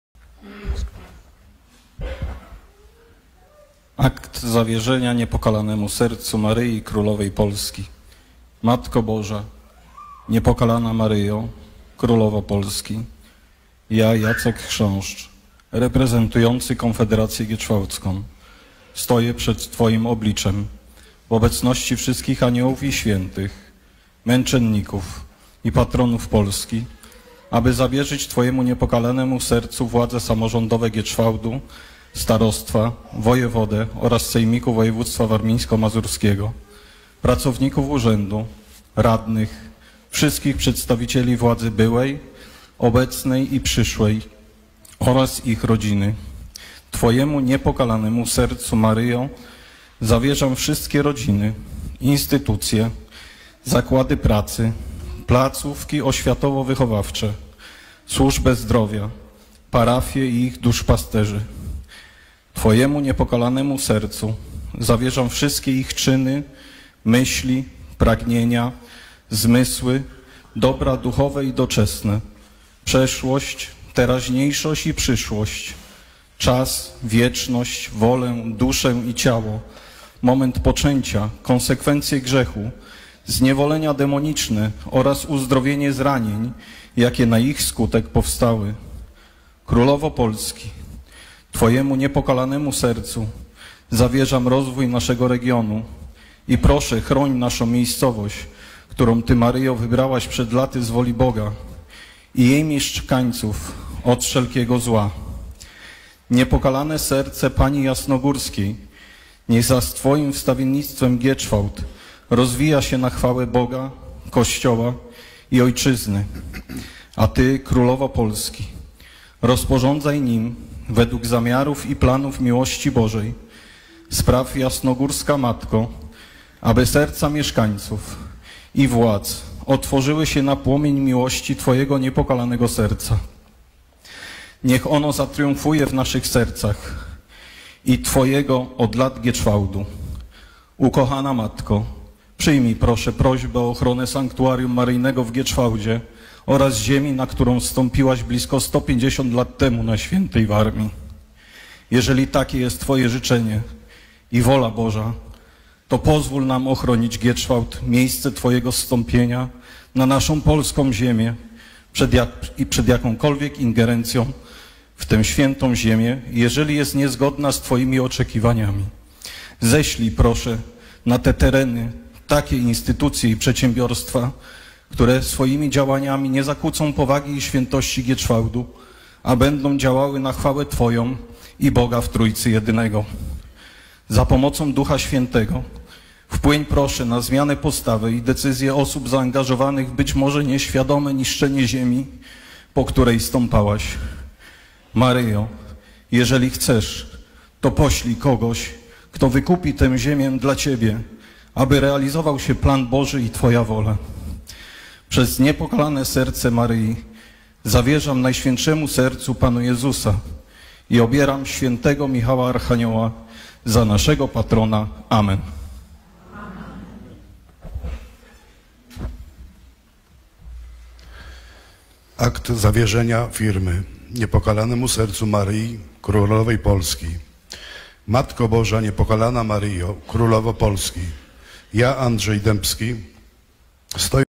Publiczne Zawierzenie Gietrzwałdu przez Prezesa Fundacji Konfederacja Gietrzwałdzka Jacka Chrząszcza
Z wielką Nadzieją i wdzięcznością informujemy, że w minioną 1️⃣-szą Sobotę Miesiąca (6.04) dokonano Publicznych Zawierzeń Gietrzwałdu i jego aktualnej sytuacji. Zawierzeń dokonano w Sanktuarium Św. Józefa w Kaliszu, Na Jasnej Górze, w Warszawie podczas Publicznego Męskiego Różańca oraz w samym Gietrzwałdzie.

Matko Boża, niech się dzieje Twoja Wola!

Fundacja Konfederacja Gietrzwałdzka

Konfederacja Korony Polskiej